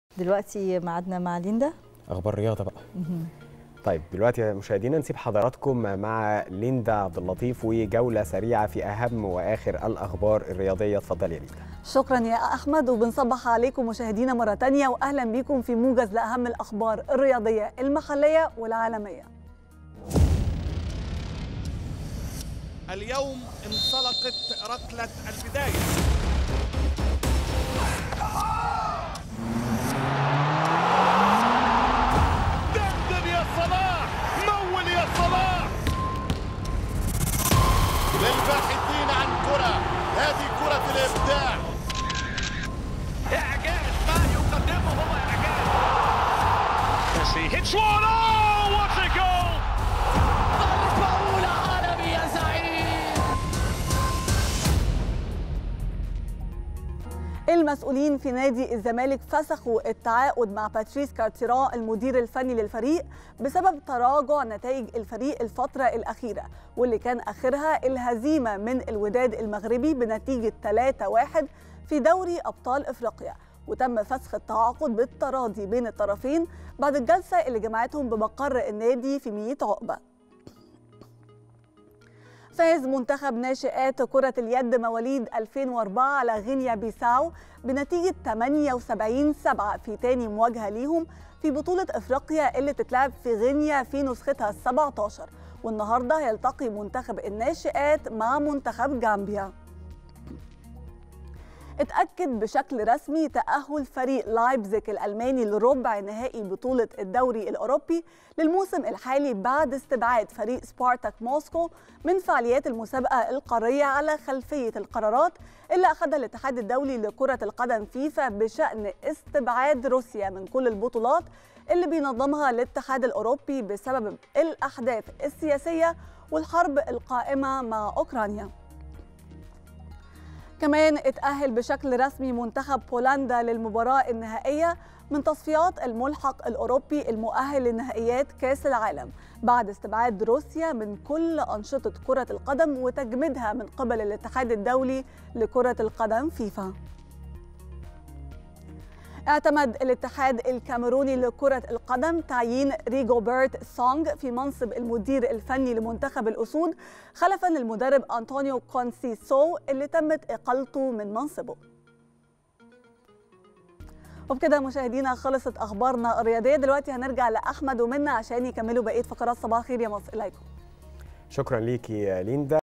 رسمياً بولندا تتأهل إلى نهائي ملحق تصفيات كأس العالم بعد استبعاد روسيا.. وأهم الأخبار الرياضية
صباح الخير يا مصر | رسمياً بولندا تتأهل إلى نهائي ملحق تصفيات كأس العالم بعد استبعاد روسيا.. وأهم الأخبار الرياضية